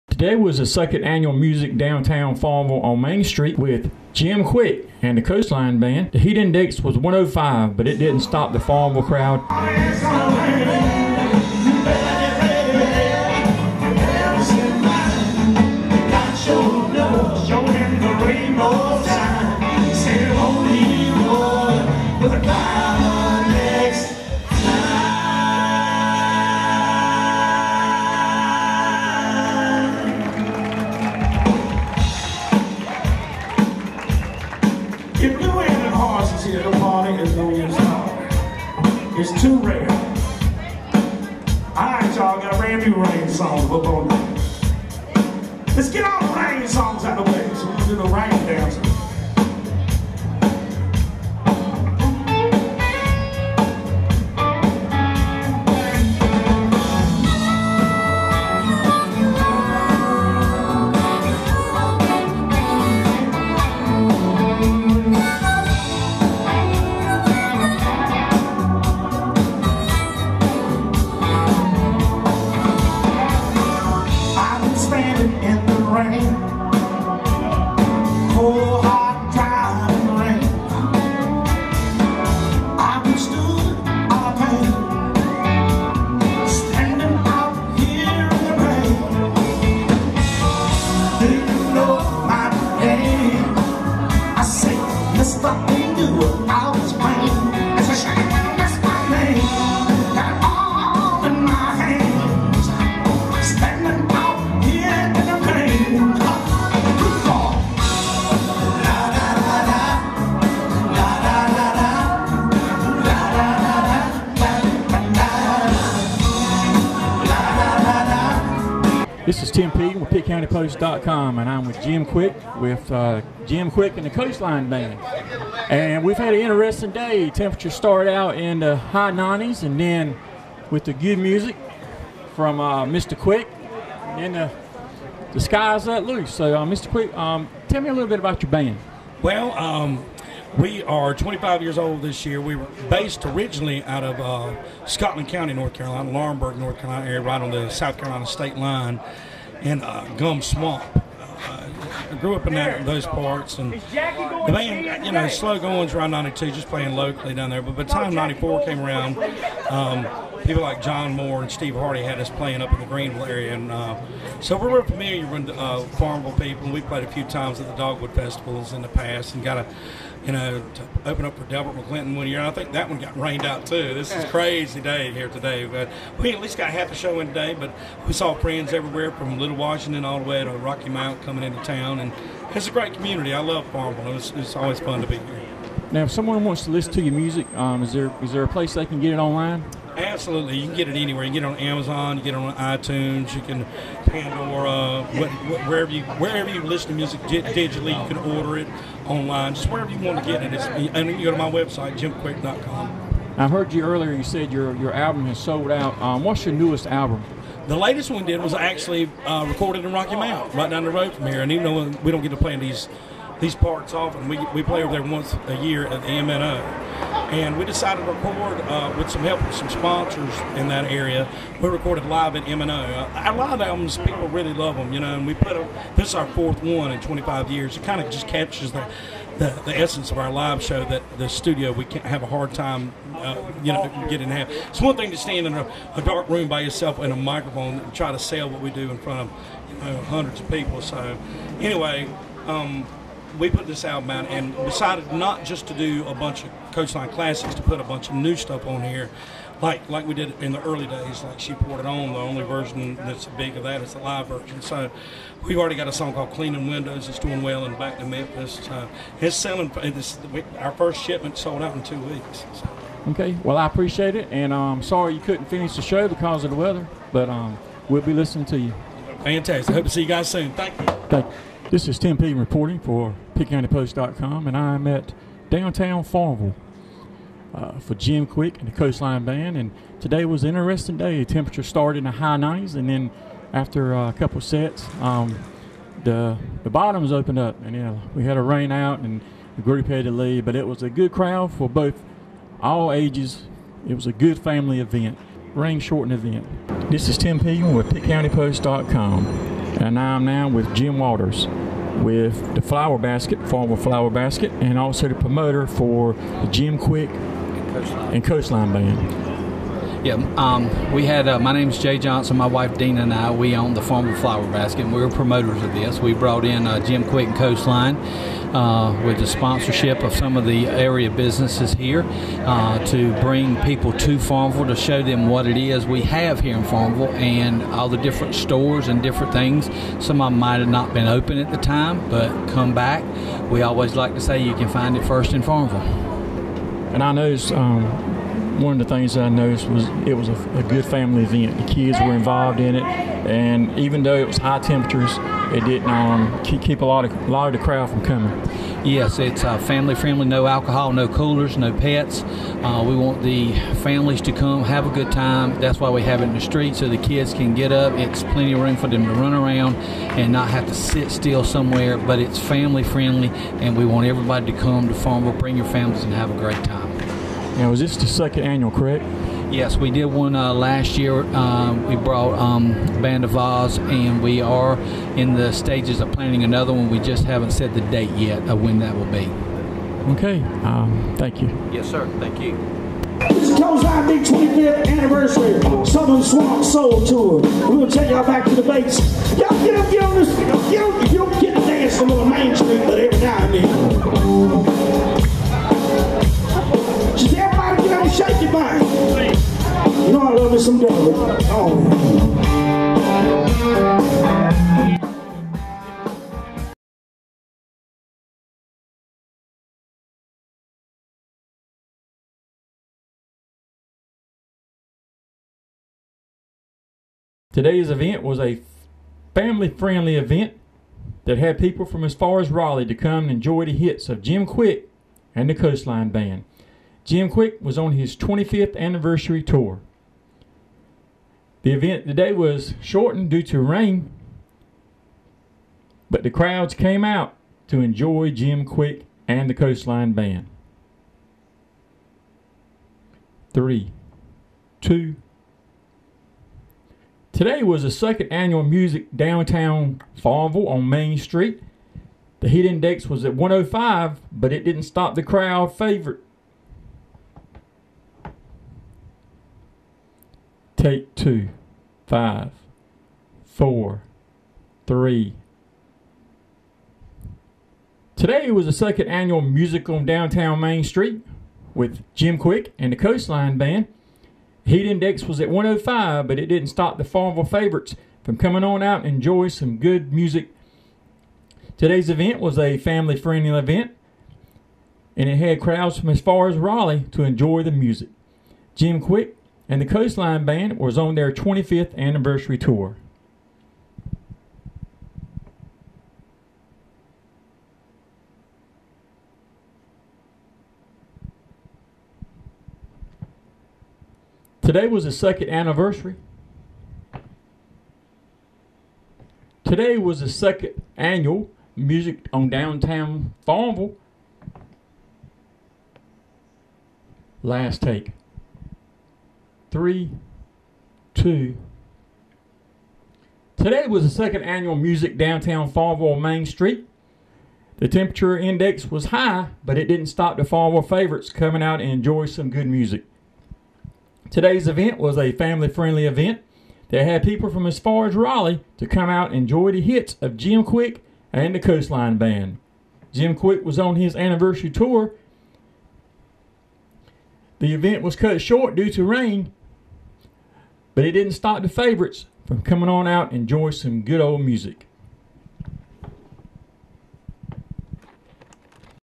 2nd Annual Music on Downtown on Main Street Farmville, NC
Jim Quick & Coastline Band at 2nd Annual Music Downtown on Main Street Farmville, N.C.